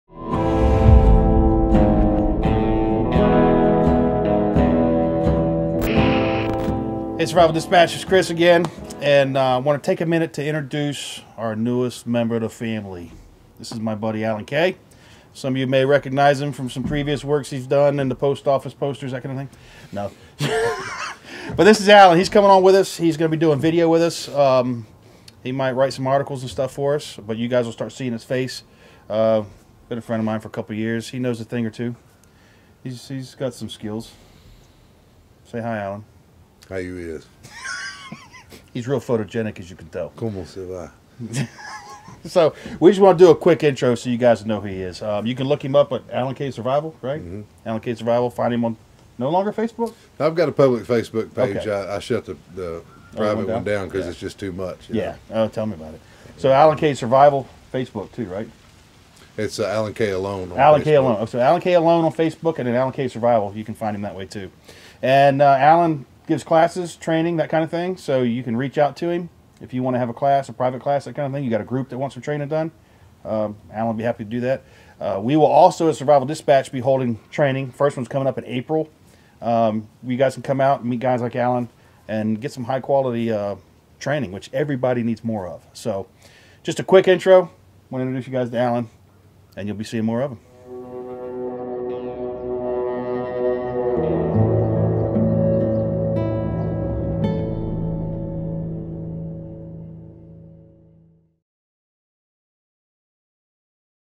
Who is Alan Kay?
Chris Weatherman gives you little background on the newest member of the team. Alan Kay.

Want more survival tips and training each month?

Become a Survival Dispatch Insider...you can try it out for 14-days for FREE.